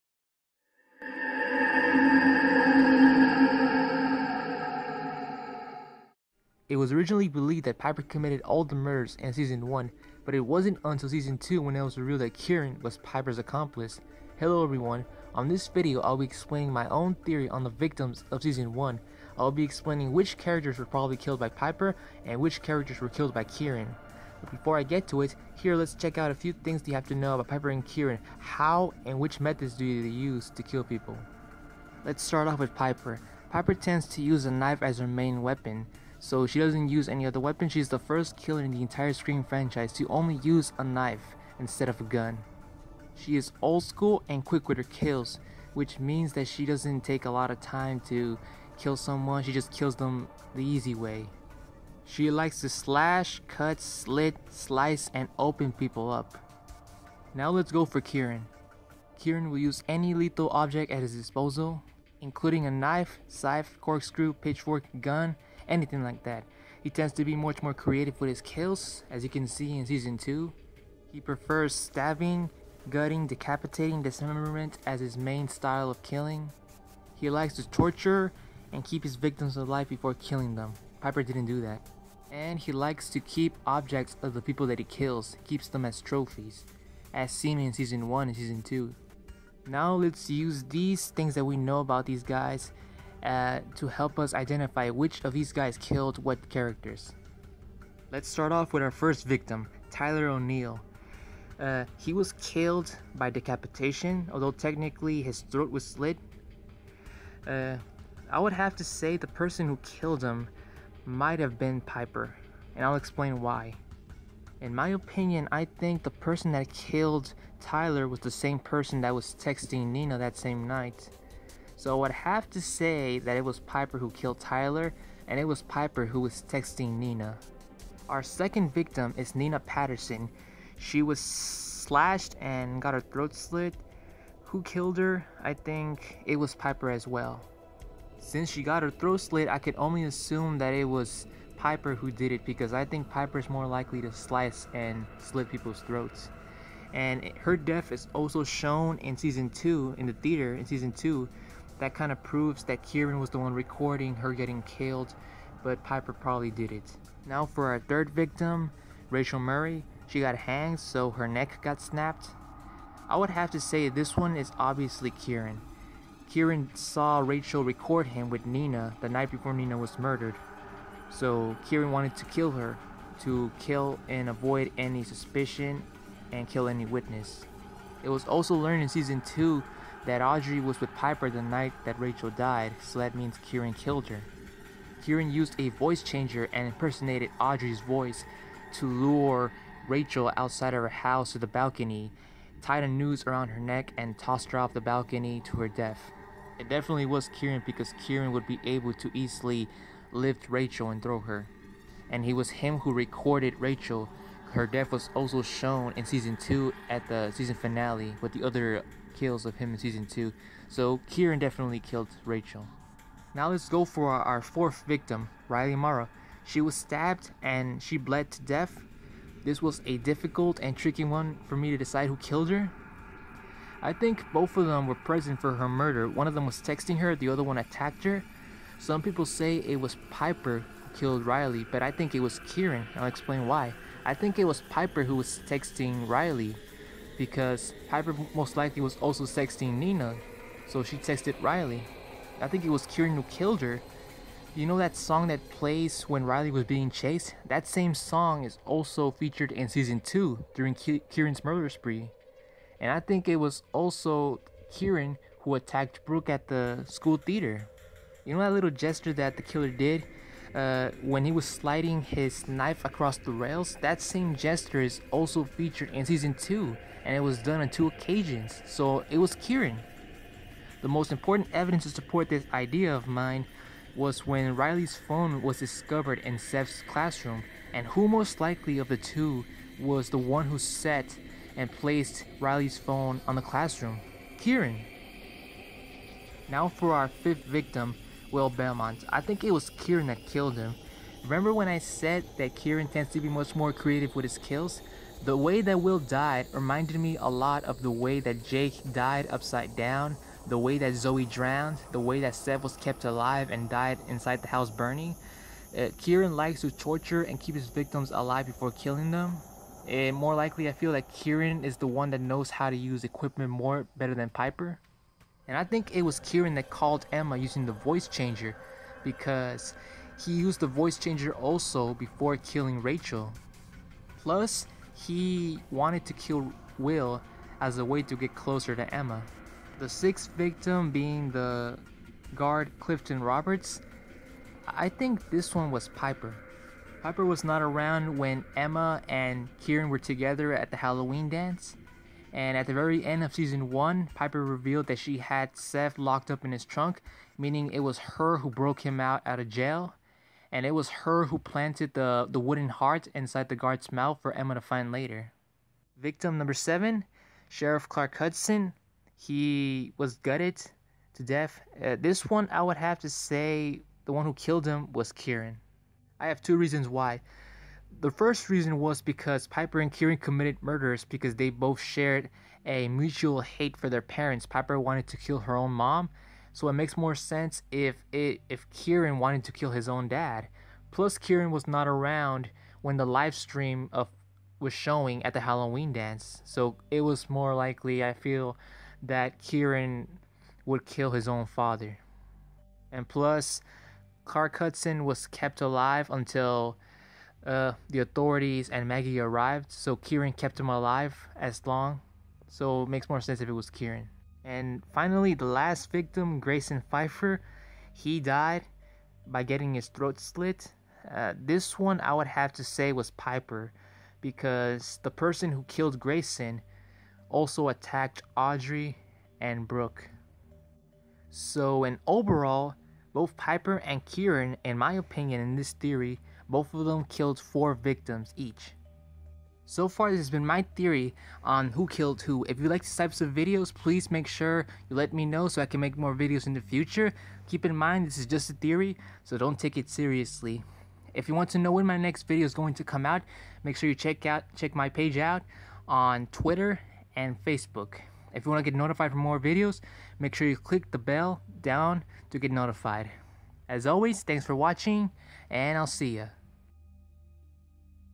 Piper and Kieran Who Killed Who? (Theory) (Season 1)
My theory on which characters were killed by Piper and Kieran accordingly.

For video updates,